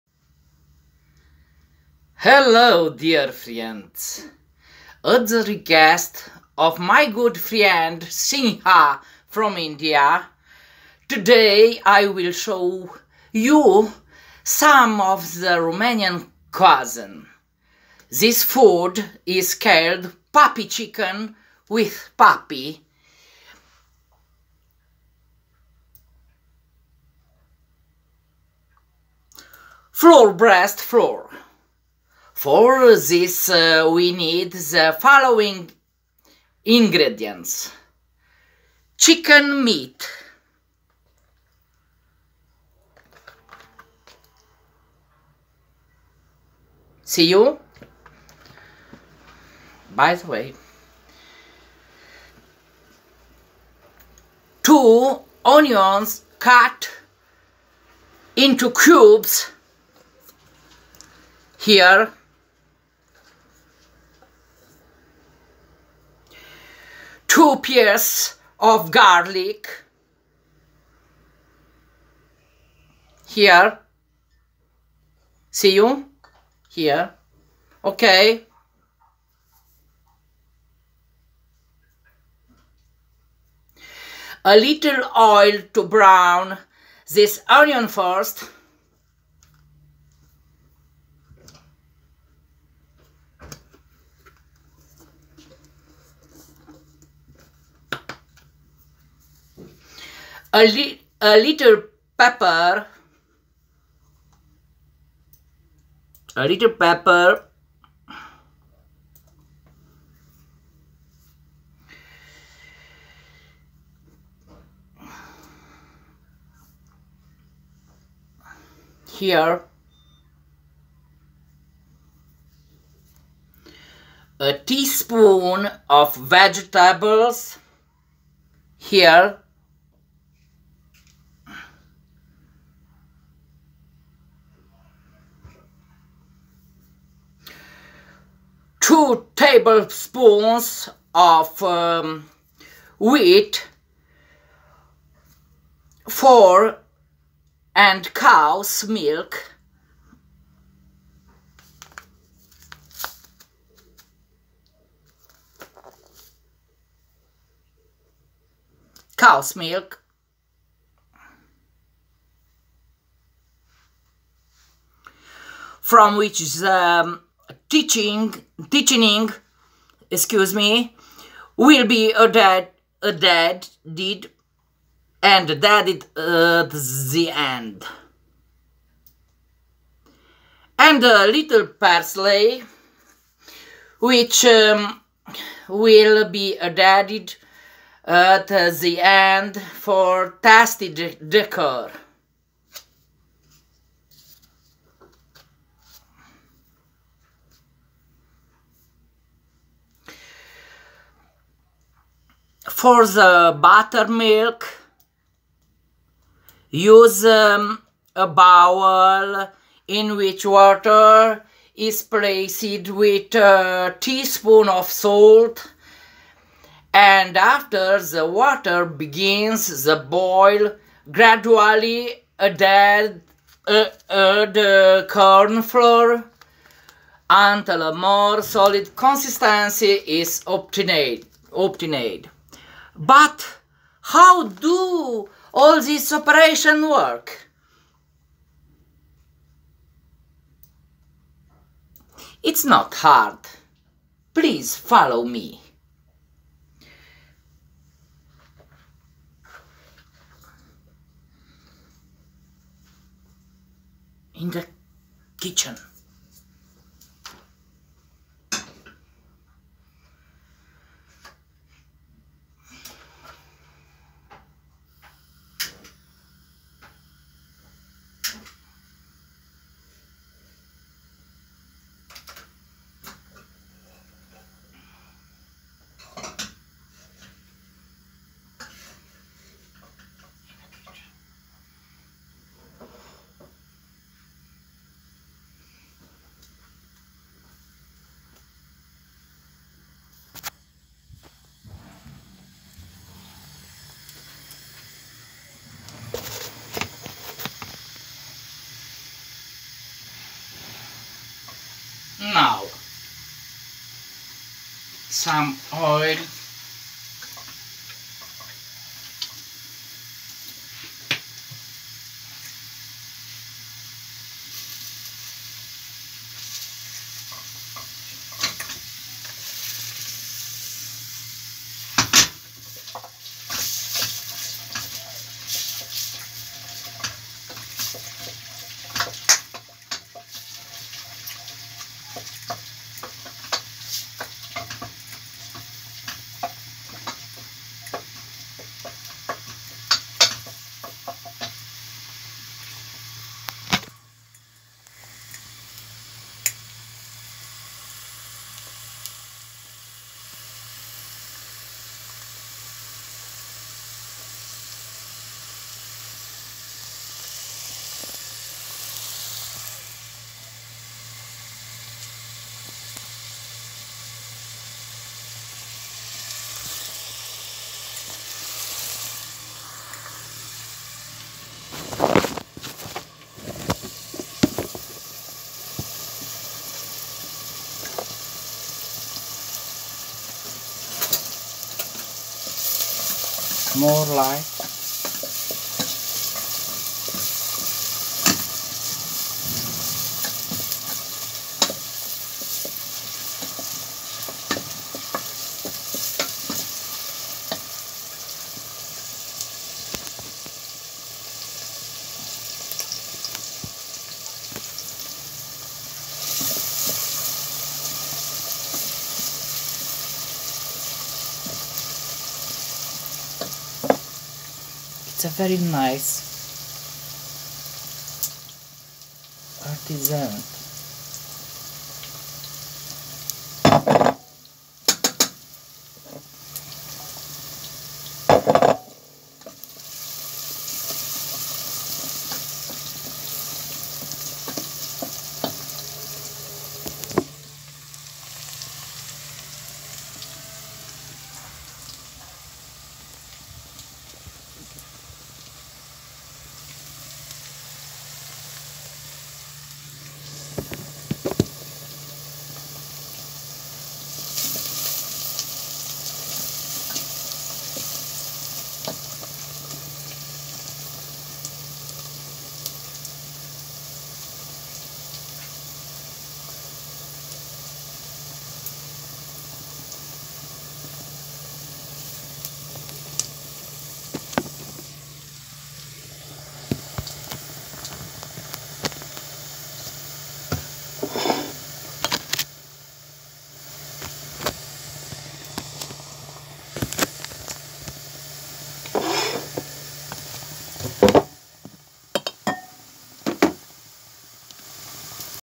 Romanian poet friend Sorin Braileanu in the Kitchen
Hello and I found you, dear friends!
At the request of my good friend Sinha from India, today I will show you some of the Romanian cuisine! This food is called PUPPY CHICKEN WITH PUPPY (FLOUR BREAST FLOUR).
For this we need the following ingredients:
- chicken meat;
- two onions cut into cubes,
- two pieces of garlic,
- a little oil to brown the onion first,
- a little pepper,
- a teaspoon of vegetables,
- 2 tablespoons of wheat flour and cow's milk from which the thickening will be added and added at the end,
- and a little parsley, which will be added at the end, for tasty decor!
For the buttermilk, use a bowl in which water is placed with a teaspoon of salt, and after the water begins to boil, gradually add corn flour until a more solid consistency is obtained!
But how do all these operations work?
Ready...
Good appetite!
Have a nice day and please don't forget that we are what we eat and what we drink, which is why we need to feed as naturally as possible!
Only good for all ... I'm Sorin and I kissed you ... God help!" Romanian poet Braileanu Sorin

And its an amazing video, my dear friend, I thankyou from the bottom of my heart! Hope all you friends will like it--its not just a cooking video but a poet volunteering to display and share some culinary skills, and the results are amazing, I can say without tasting! My deeply felt thanks to the amazing poet, whose poems also I will present, later, but first his introduction Although he is of Banat origin being born in Timișoara, a big city in Romania, he was little adopted by the Transylvanians, who grew up and educated how they could do better and more. Sorin - Ionel Brăileanu from Cluj-Napoca, is one of the most learned people and has both military and civil studies, being a doctor of sciences. At the same time, he is the initiator and coordinator of a large national festival of folklore, customs and local traditions with the title: "Western cultural traditions - ancestral practices in a contemporary context", being fascinated by traditions and popular song. He absolutely loves the rural environment, the children and the people with the beautiful soul, the water and the ancestral land, the flowers and the animals, the warm summer rains, the rays of the Sun and the wind, he loves his family, the mother country and the tricolor flag! He has many talents ... he is an exceptional teacher, poet and prose writer, journalist, organizer of folk events, presenter and performer of popular music! Regarding the poetic talent, he is the author of a superb volume of love poetry, which is titled: "Between hazards and destiny" published by the AcademicPres Cluj-Napoca Publishing House in 2016. "There are love poems that talk about meetings and reunions. fecund, about loss and longing, death and the need for serenity. The lyrical journey, although uneven in stylistic forms and experiences, reveals and unexpected depths of feeling, contradictory disturbances and desires, haunts and rejections - all as signs of a destiny of the clay being ”, says with joy and emotion, Mrs. Emilia Bia, in the preface to this literary work. Through his entire activity, through his studies, through his poems and songs, through his popular port, this great Romanian keeps and promotes these traditional values and at the same time, he tries and manages to sit among the organizers of such events, which do not I can do nothing but rejoice and fill our hearts with the beautiful and the authentic in us.

And a poem by him, which is for a mother--when we think kitchen, food, recipie, we always think of our homes, and mums, so a tribute to a mother---and during the course of our conversation, Sorin told me this poem is inspired by a friend's mother, hope you will like it too--
A THOUGHT FOR MOM! My sadness comes over me
 I can cry again
Because tomorrow is your day
And you got into my mind.

It's your birthday nun
A spring day
You're no longer with me
A thought towards you is flying.

and how much I would like
With the palm of my face to comfort me
And tell me, "Good night!"

Oh, my God, and how much I would like
Let me see you on the threshold again
You had to wait for me
To look at me
with pleasure ...

My little girl, beautiful fairy
You went on a spring day
And everyone now takes my hand
In my soul I have bitter pain.

Mom, you didn't say a word!
You comfortably stroked my head ...
And you left with the Holy Spirit
What could I, mother, do?
I cried for a while after you
With warm baby tears
I just knew what was going on ...
 And I kept quiet!
Sorin BRĂILEANU, March, 2018 heart touching poem! Beautiful indeed!

Well, in our case, both Sorin and I have our mothers, while, we have lost our dads! The kitchen, family meals remind us all of both our parents, and I want to take a moment to remember my dear dad, for whom, I wish to read this poem!